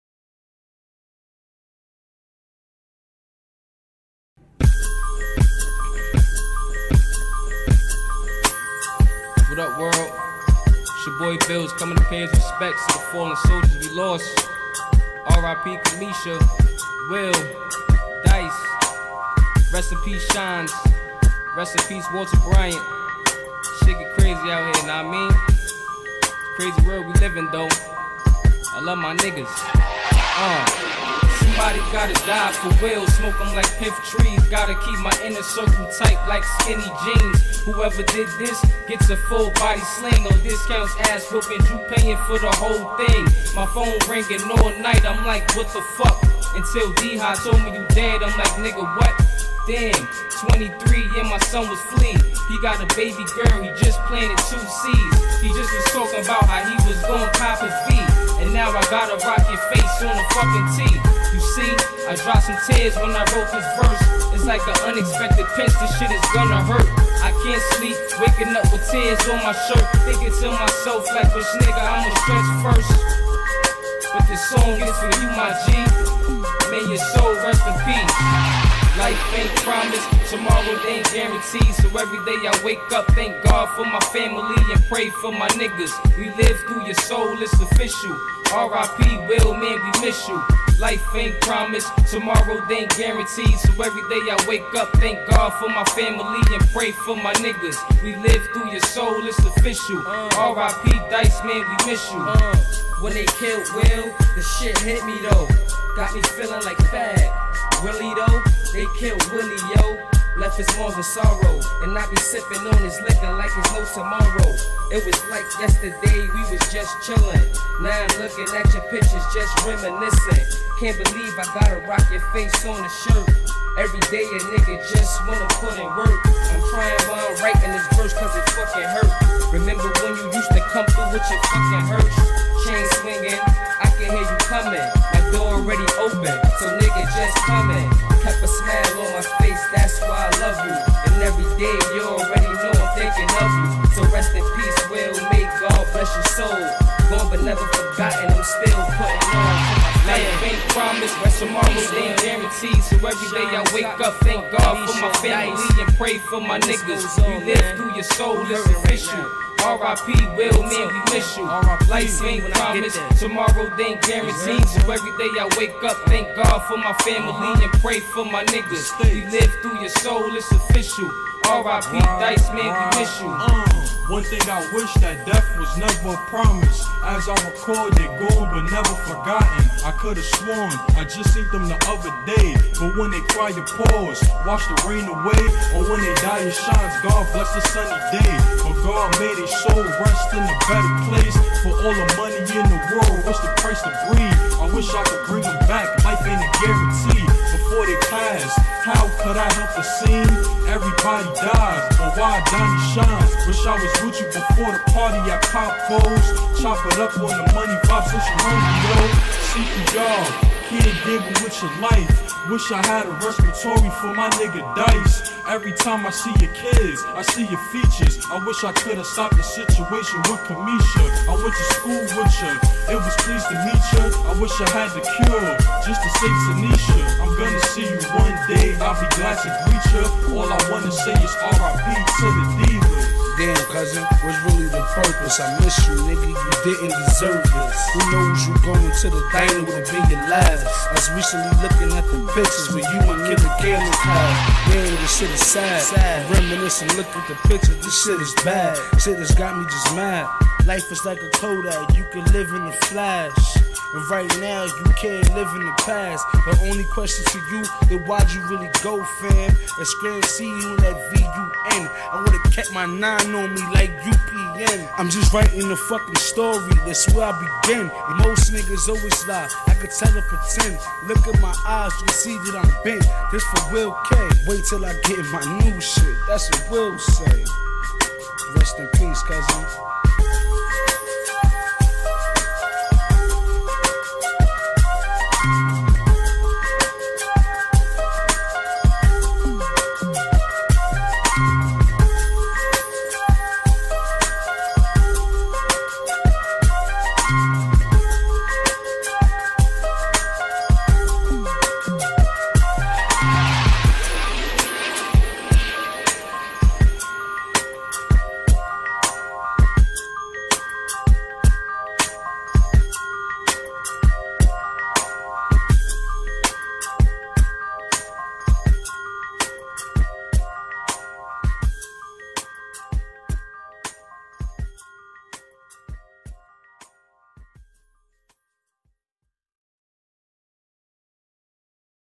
R.i.P Tribute
BIllz, Bucs, Ruga and mova do a R.I.P Tribute to our fallen loved ones